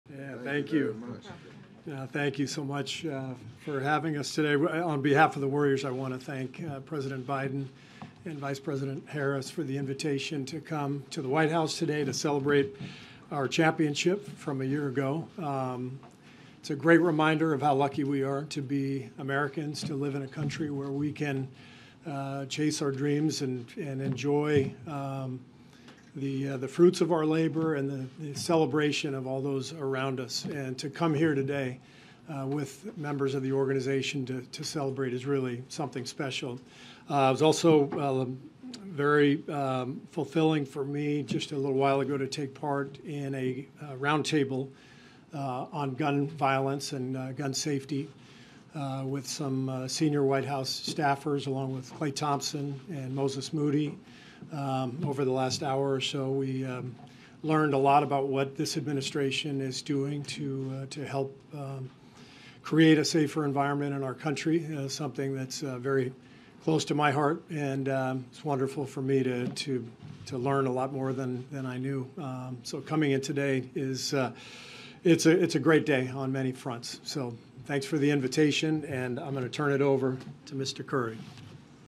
NBA Coach Steve Kerr Thanks Biden For First White House Since Snubbing Trump’s Invitations
On Tuesday, Steve Kerr, head coach for the Golden State Warriors of the NBA, joined the press briefing during the team’s visit to the White House to celebrate their 2022 championship win.